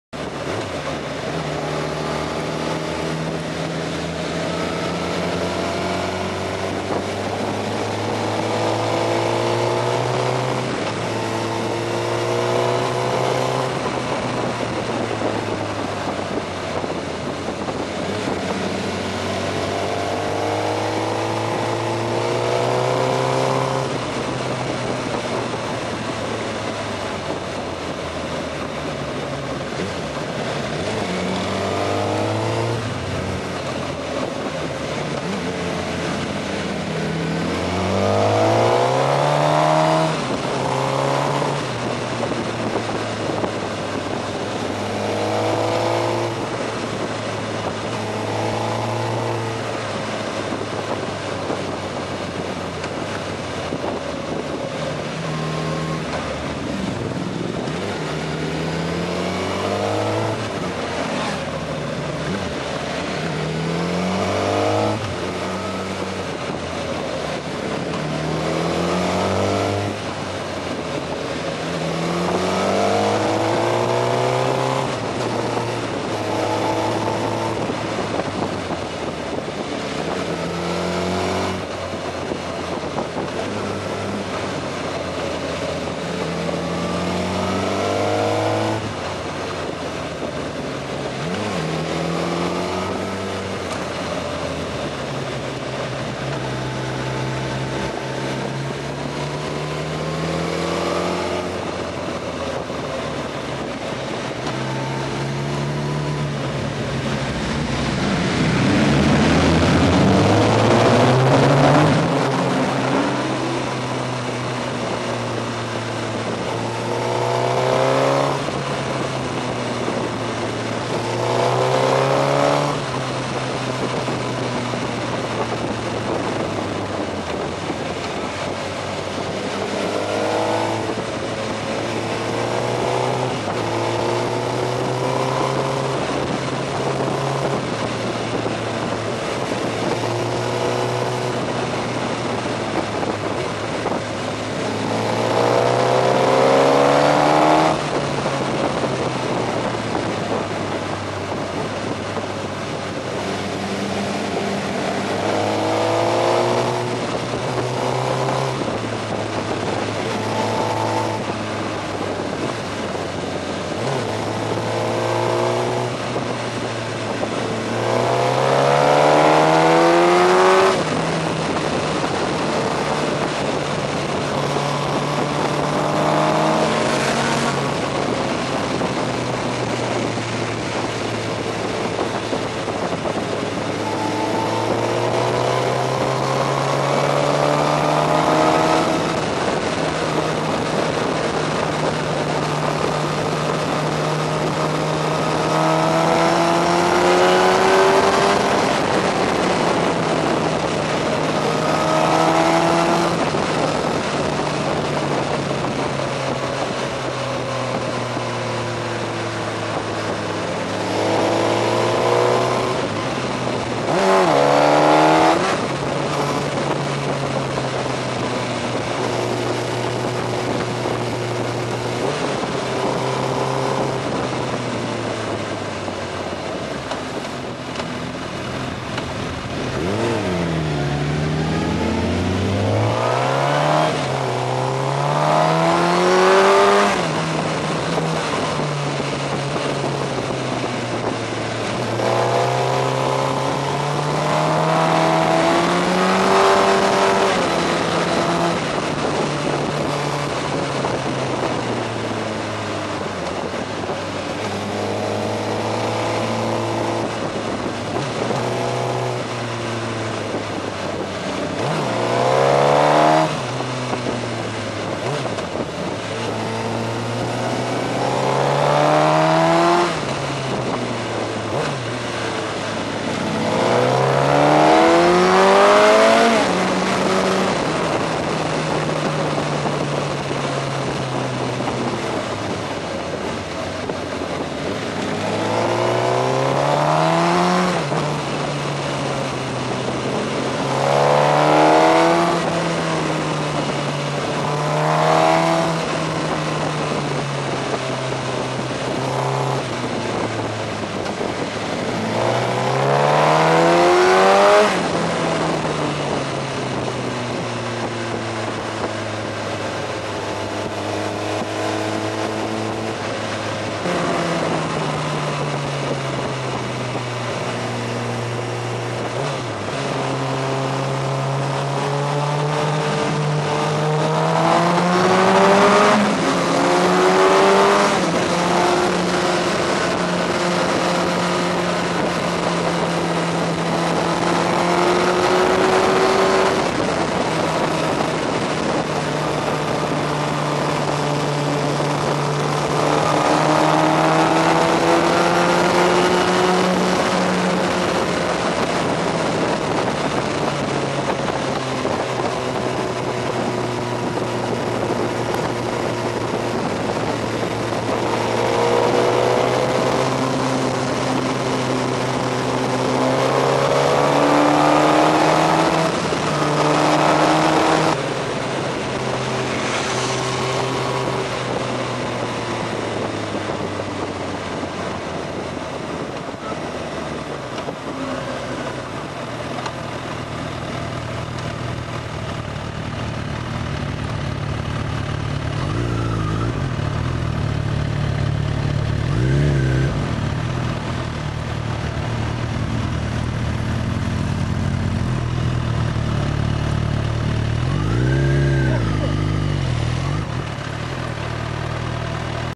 Die Sonndaachfahrer: In den Schweizer Alpen - Simplon Pass 2009
Eine Fahrt mit den Sonndaachfahrern durch die schönen Schweizer Alpen zum Simplon Pass mit KTM 950, GSX-R 1000 (Kamera) und Yamaha FAZER 1000.